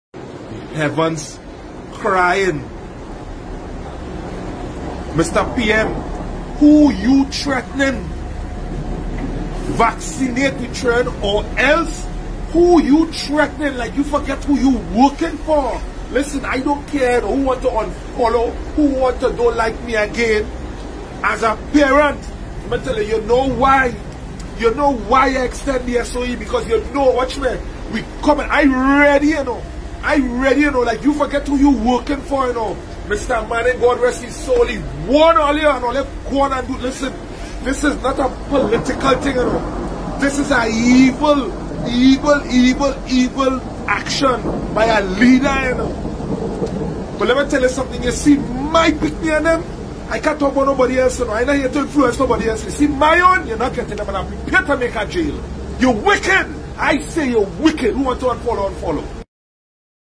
🇹🇹 RESPONSE TO T&T PM'S THREAT TO PARENTS!
This videoblogger is very upset by the T&T PM's recent threat to parents about jabbing the children against parents wishes and asks the PM if he has forgotten who he was elected to work for.